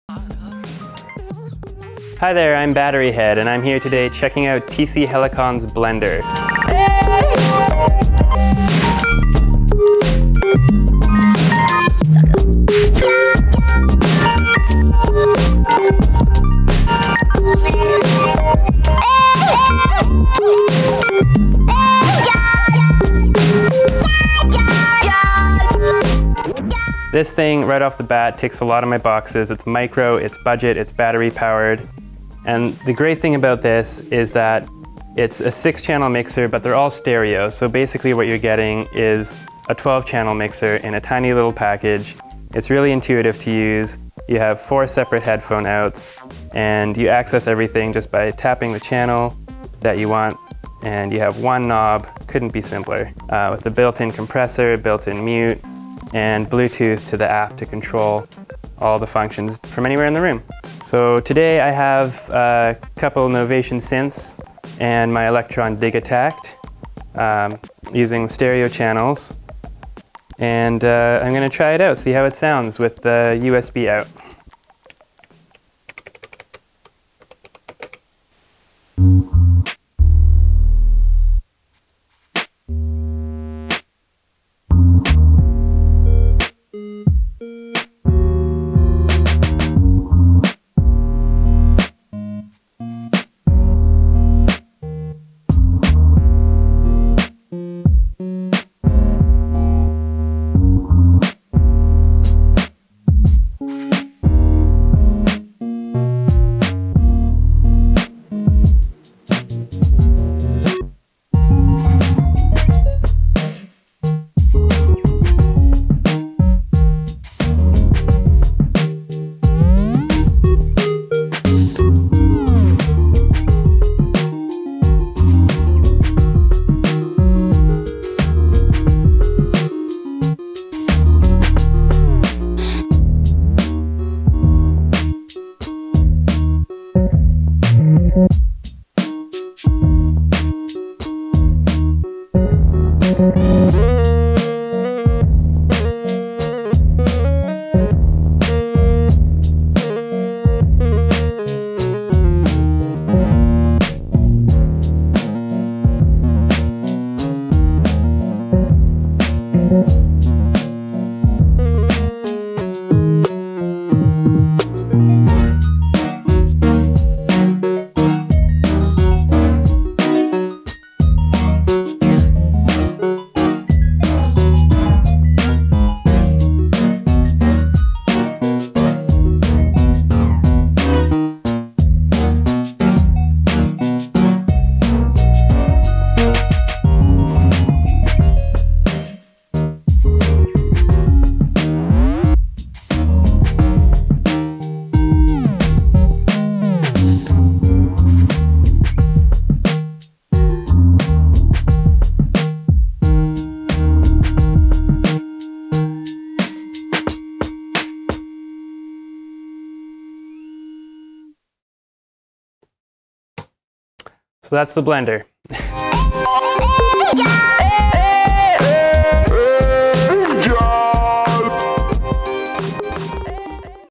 Studio Session: Batteryhead with Blender & Synths
Batteryhead is a synth musician and performer in Canada who has been on the lookout for the perfect portable mixer for his synth performance setup.

So we invited him to the studio to check out Blender! Blender is the perfect addition for synth & EMD artists who want total control over the mix of their various instruments.

With Blender, Batteryhead connects the synths he is using to the 6 stereo inputs, and connects to the Blender app for managing the mix at his fingertips. With this simple setup, he's ready to record up to 12 channel multi-track straight to a DAW from Blender by connecting from the USB output!

In this Video, Batteryhead is using two Novation Synths and one Elektron Digitakt, and the audio you hear has all been recorded through Blender to a DAW.

Batteryhead on Instagram:  @batteryhead.music